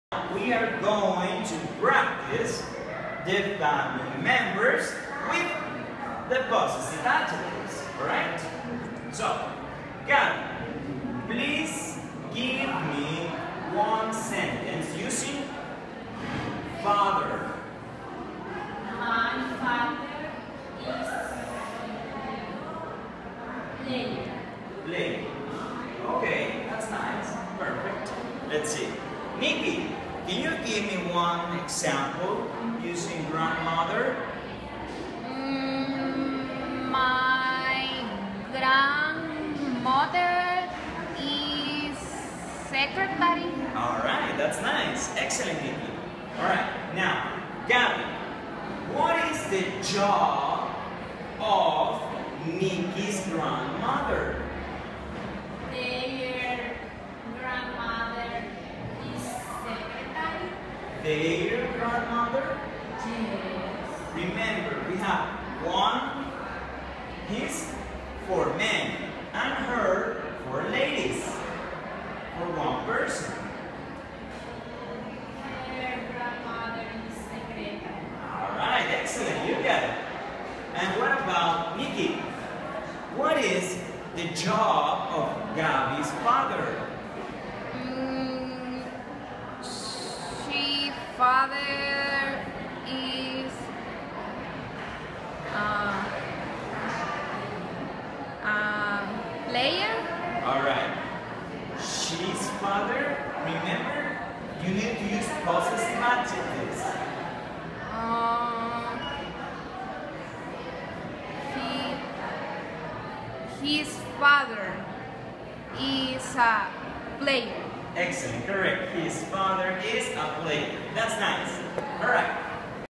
Corrective Feedback
This is an example how to correct mistakes in a lesson class .